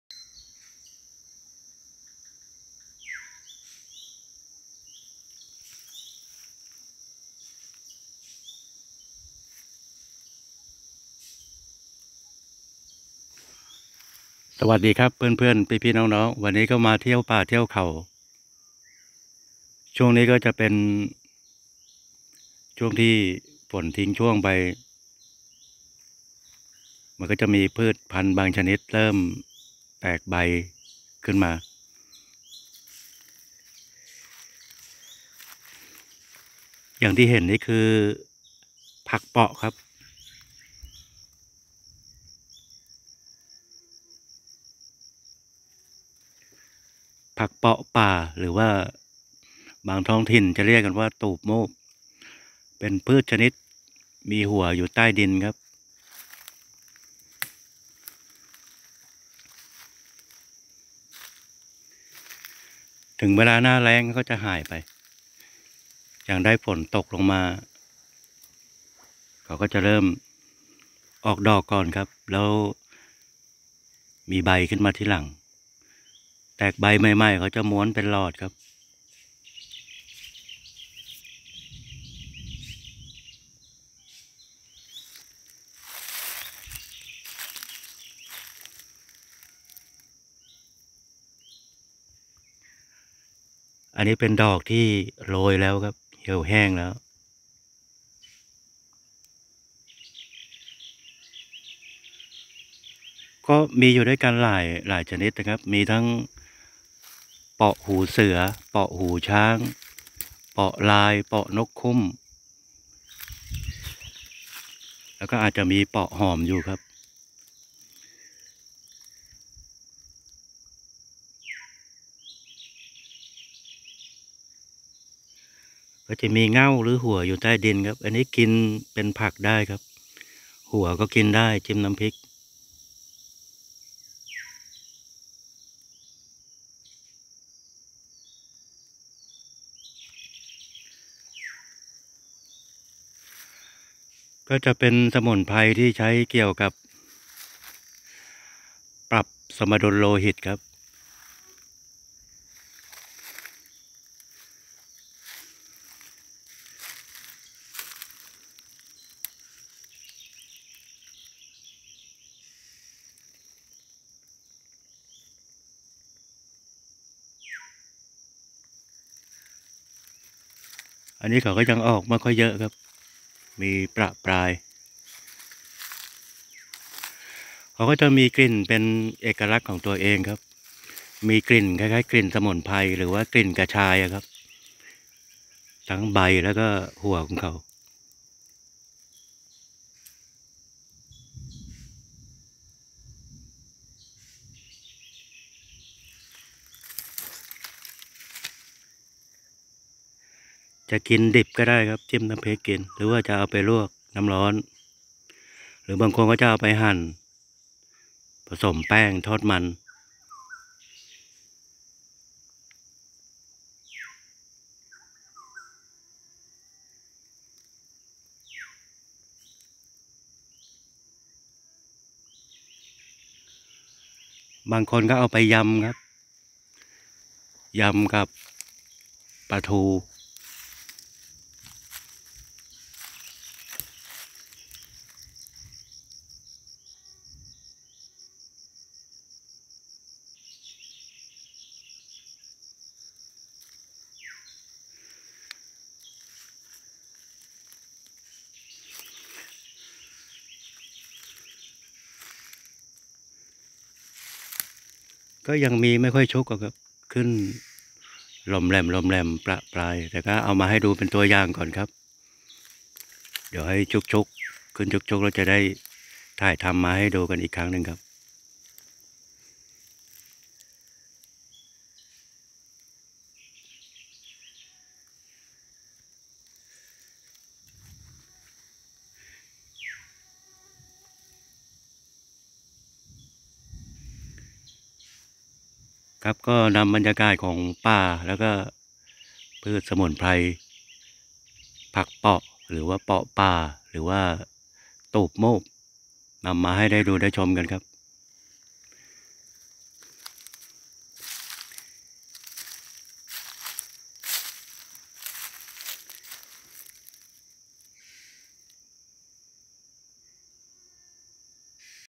สมุนไพรตูบหมูบ(ต้นเปราะ) 🌱🌱🌱
ต้นเประ บางท้องถิ่นเรียกตูบหมูบ เป็นพืชล้มลูกขนาดเล็ก มีหัวใต้ดิน หน้าแล้งจะแห้งหายไปพอได้ฝนจะงอกงามขึ้มมาใหม่ ใช้กินเป็นผักสมุนไพรได้ทั้งหัวและใบมีกลิ่นหอมแบบสมุนไพรคล้ายกลิ่นกระชาย มีสรรพคุณช่วยปรับสมดุลโลหิตได้ดี🌱🌱🌱